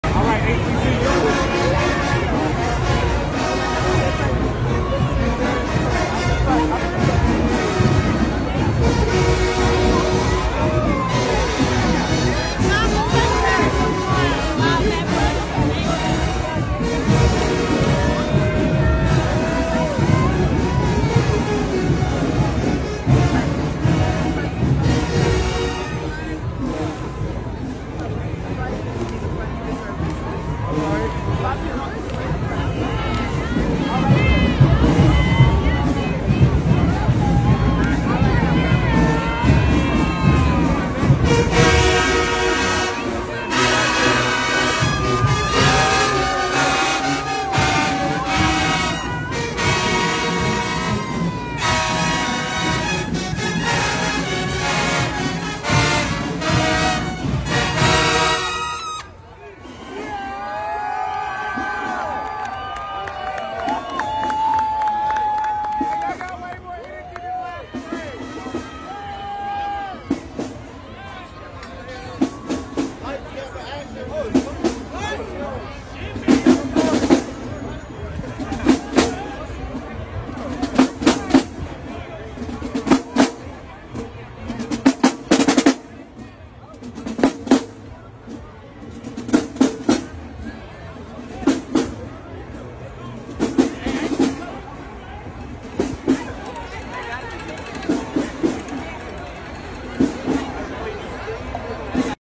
Talladega College Marching Band Bacchus Parade 2024
Talladega College Marching Band Performing IN Krewe Of Bacchus Parade 2024